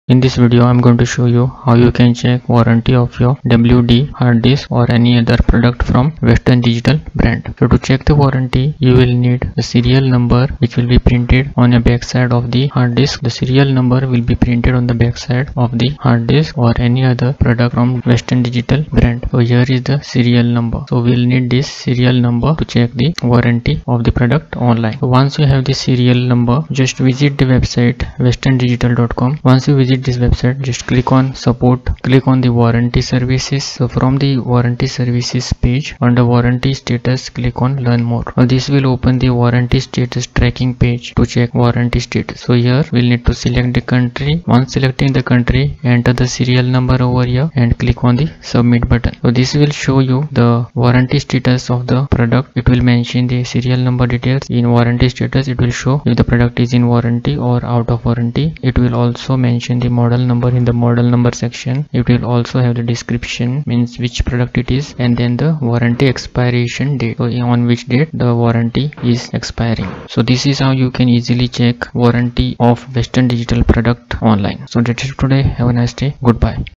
How to Check WD Warranty | Western Digital Warranty Check
This video shows how you can check WD (Western Digital) Warranty Online to know if the product is under warranty or out of warranty and exact expiry date of the warranty.